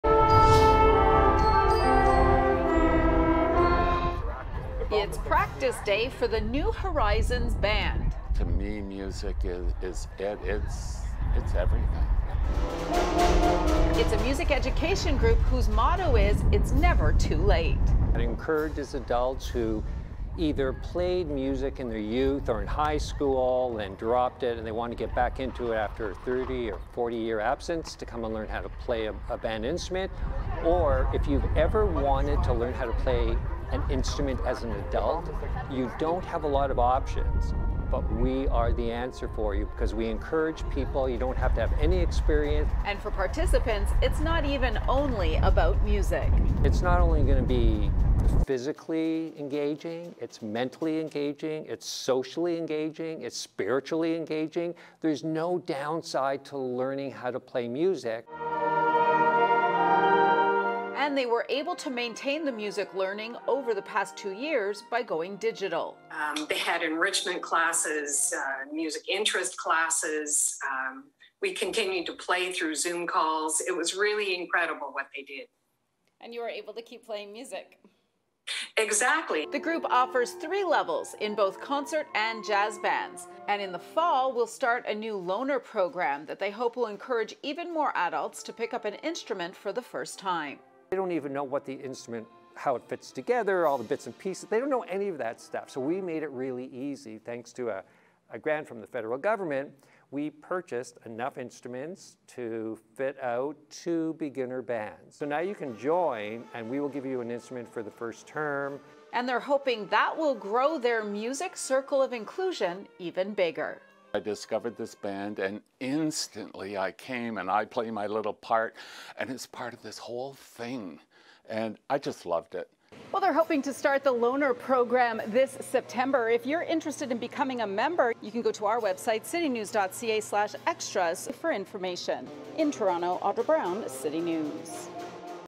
A band for adult music students
New Horizons Band says it never too late to learn to play an instrument. Videographer Audra Brown joined the band that teaches hundreds of first time adult music students.